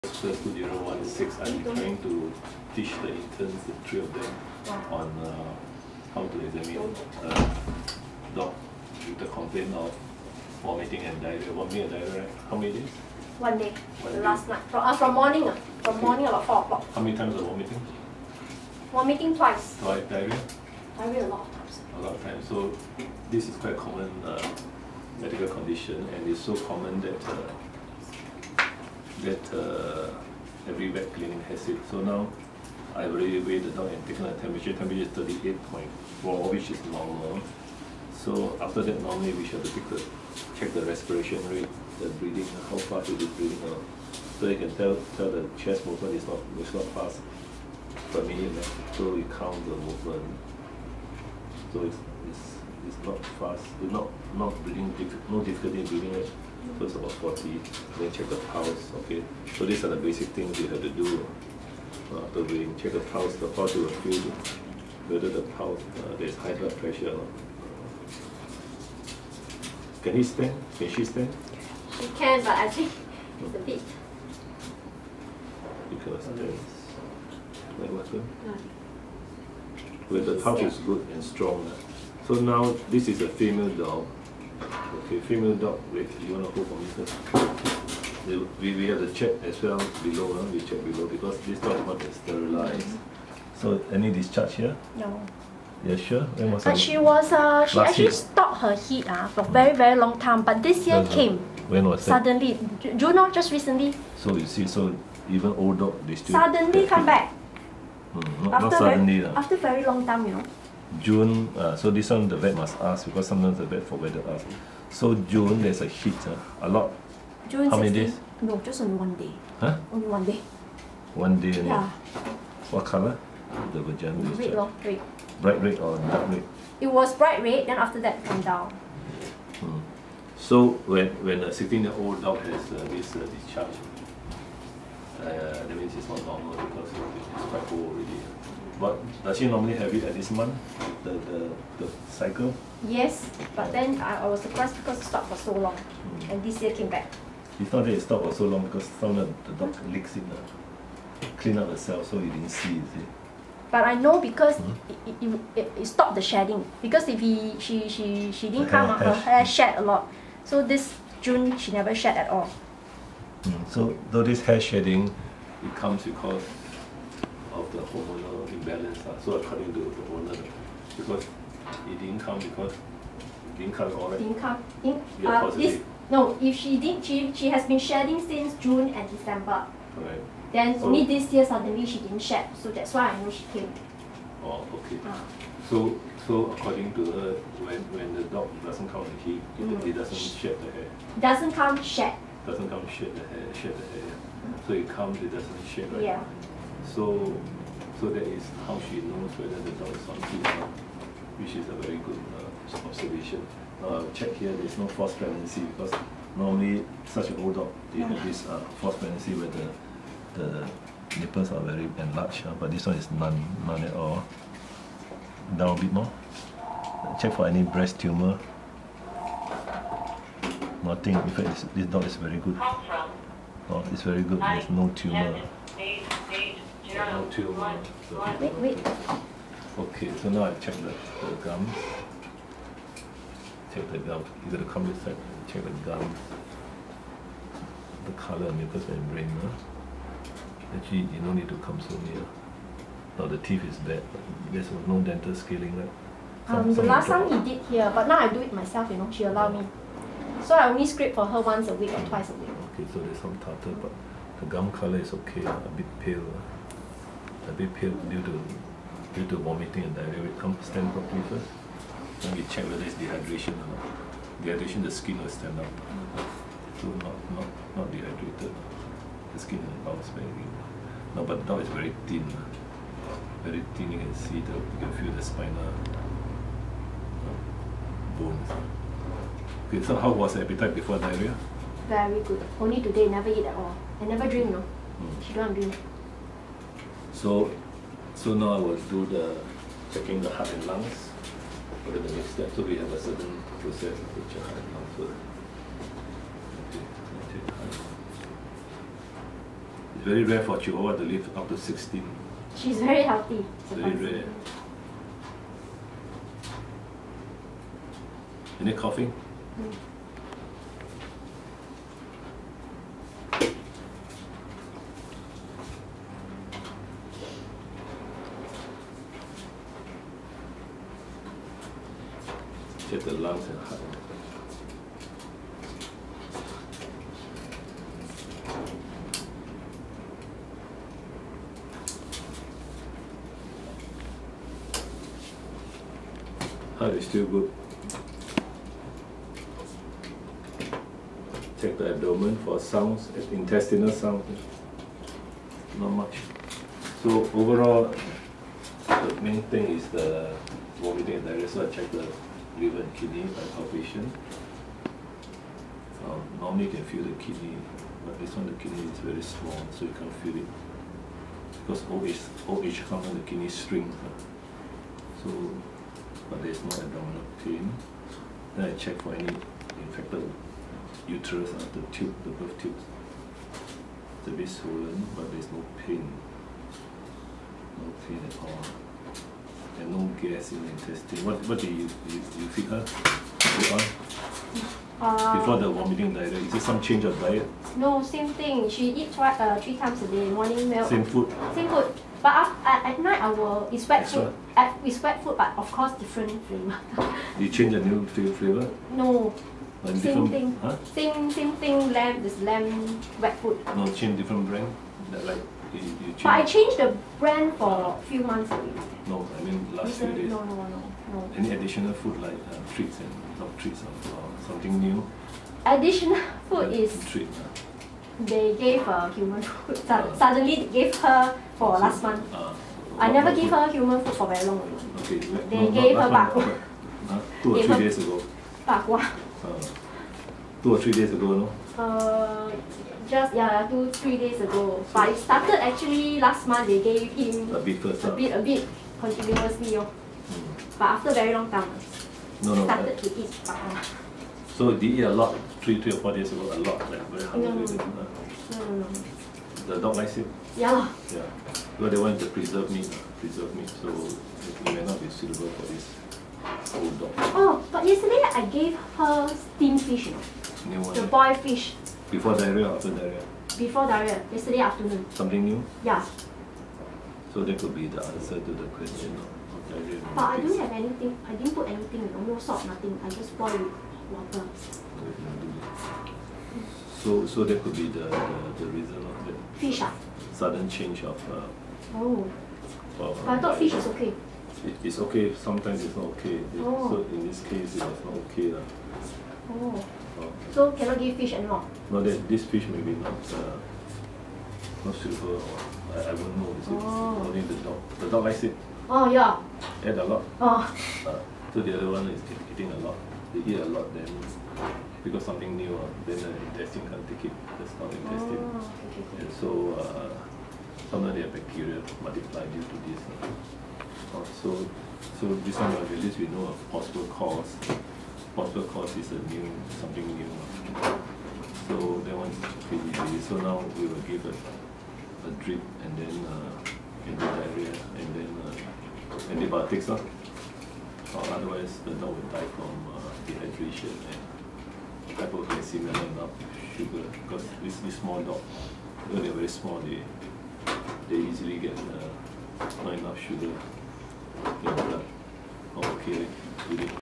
A 16-year-old chihuahua has vomiting and diarrhoea Pt 1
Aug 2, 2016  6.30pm
Emergency vomiting and diarrhea case

Case study for 2 Sec 4 interns from RI and 1 Sec 3 CHIJ interns
Vomiting 2 x and diarhoea many times
On heat in June 2016 suddenly after a long period of no heat. Could this be a case of pyometra?
Likely diagnosis is gastroenteritis due to eating  a new food - fish. IV drip given as shown in the video to prevent dehydration and give antibiotics and anti-diarrhea directly. Blood test taken.

3 interns get to see the process of diagnosis and treatment. To review next day.